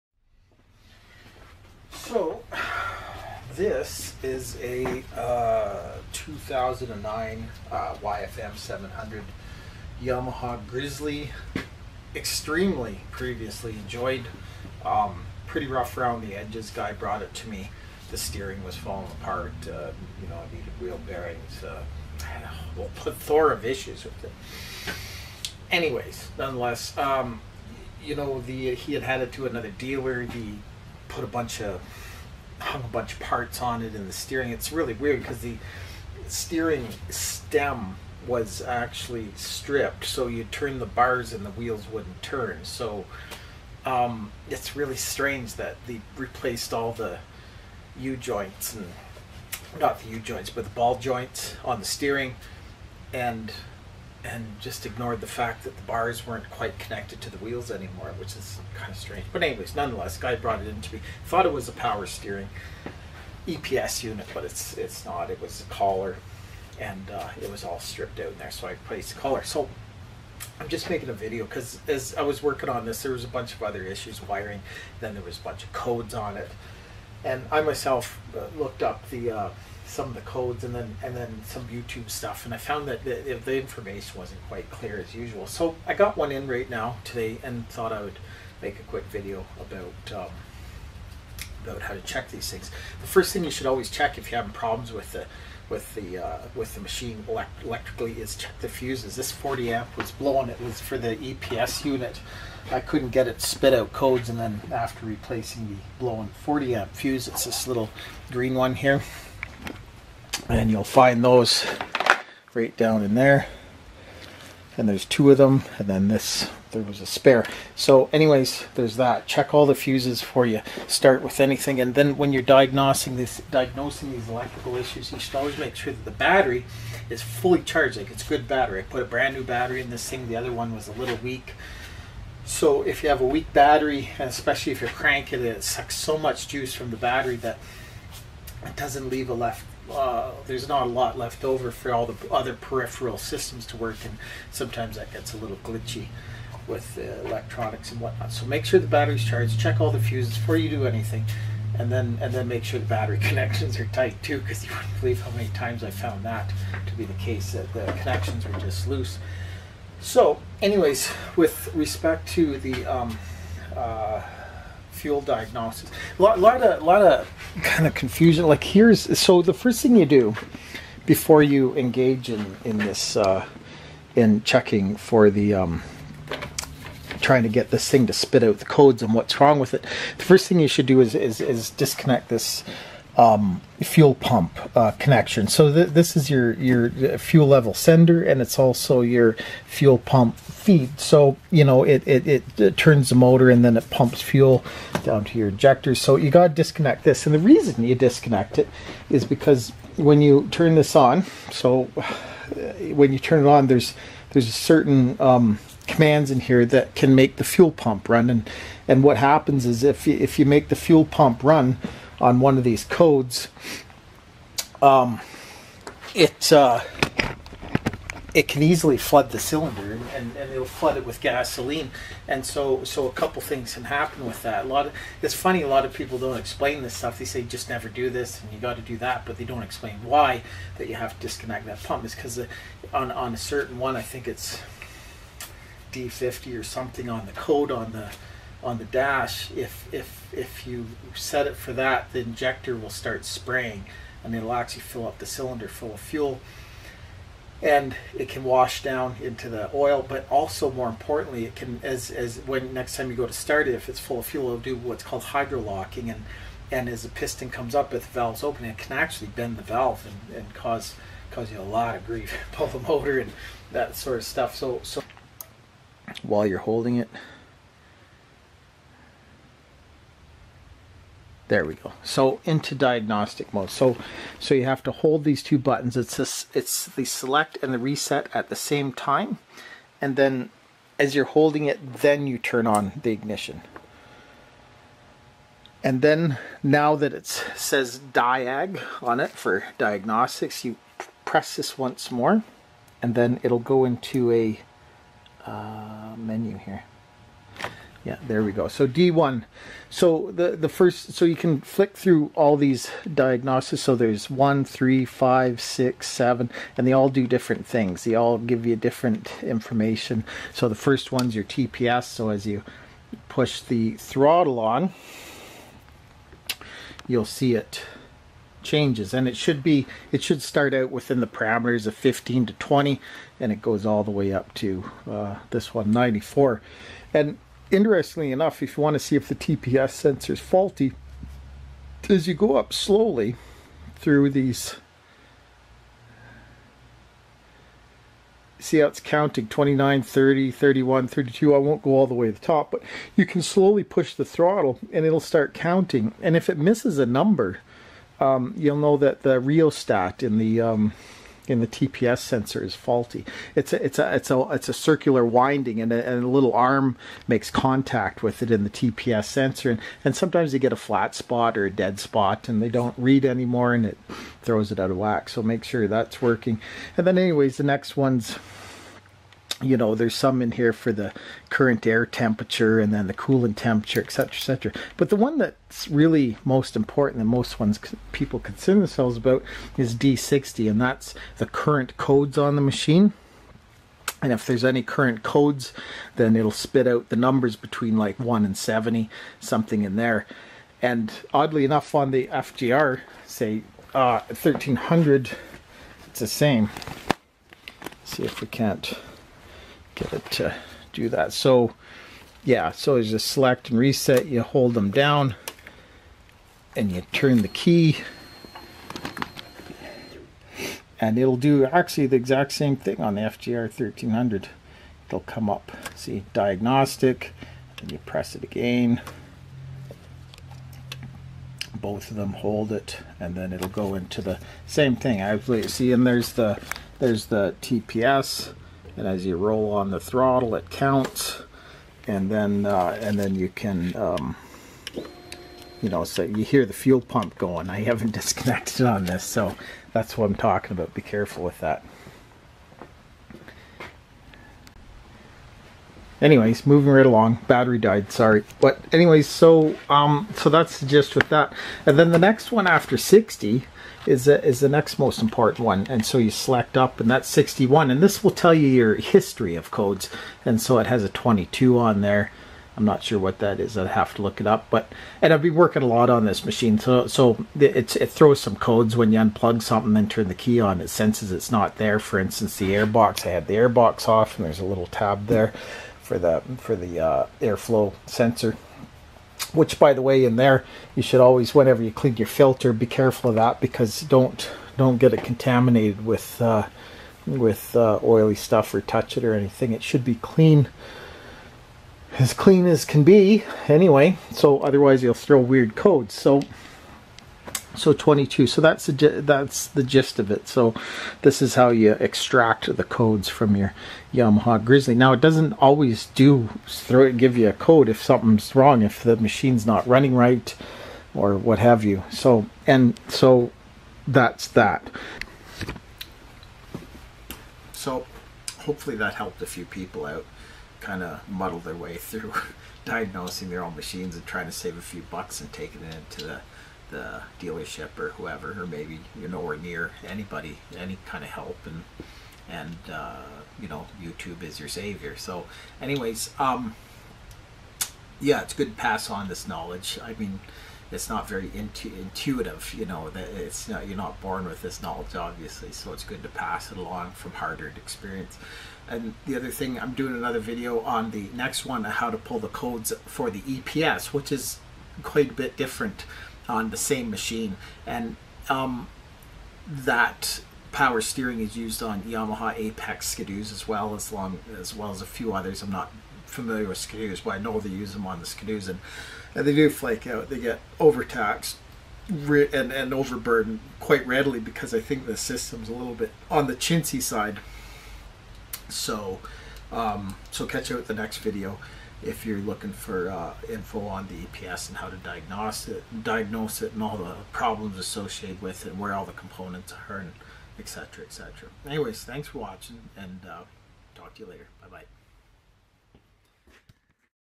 Yamaha YFM Grizzly EFI and EPS codes
A little dive into pulling Yamaha codes on YFM, YRX machines and similar, why TPS sensors fail, how to avoid destroying your engine during diagnosis, and other pearls of wisdom.